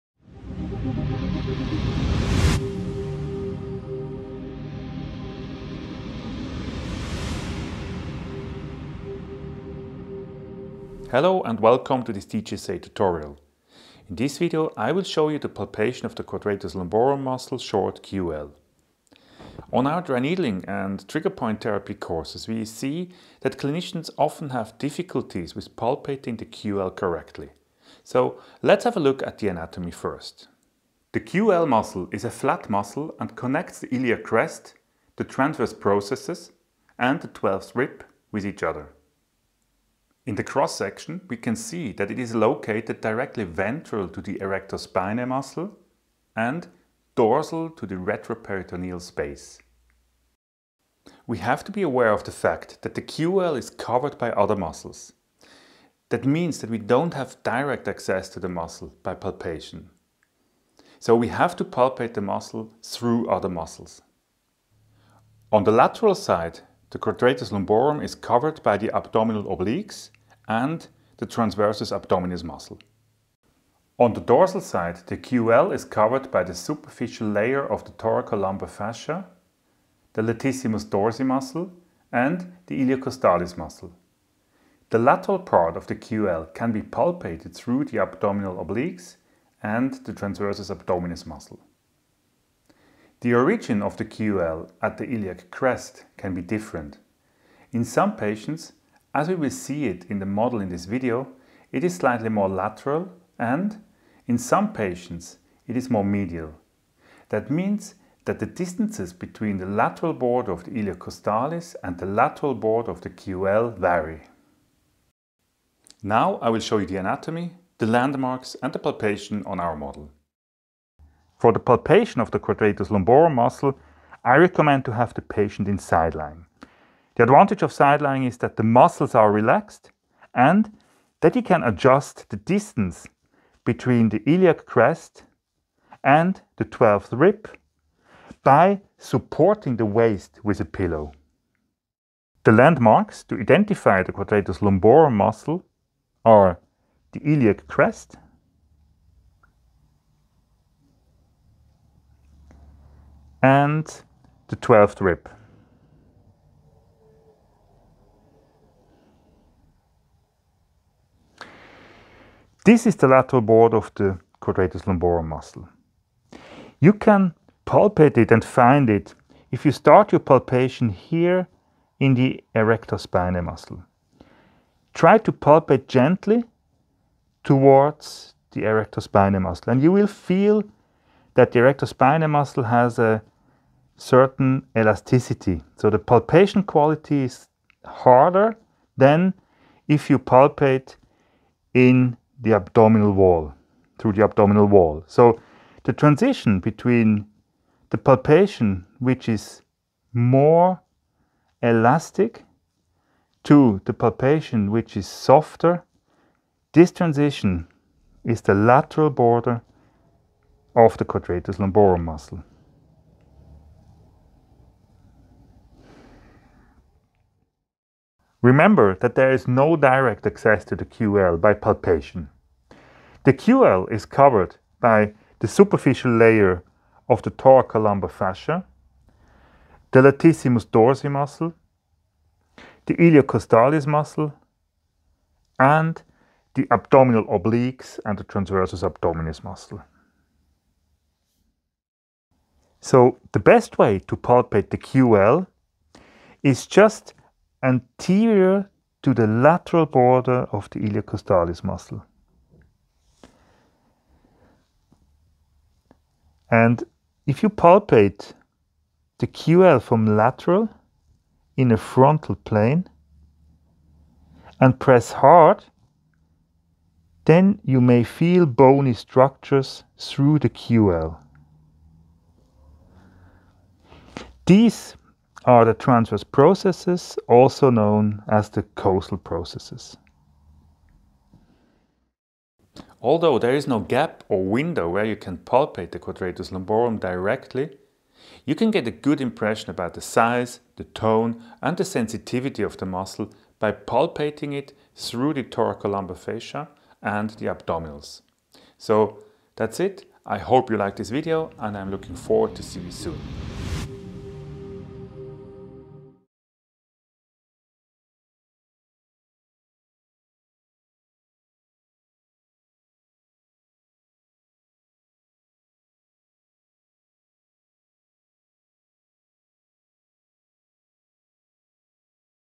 Palpation of the Quadratus Lumborum Muscle
In this video Christian Gröbli will show you the palpation of the Quadratus Lumborum muscle, short QL. In our dry needling and manual trigger point therapy courses we see, that clinicians often have difficulties, palpating the QL correctly. Here you will learn how to do it!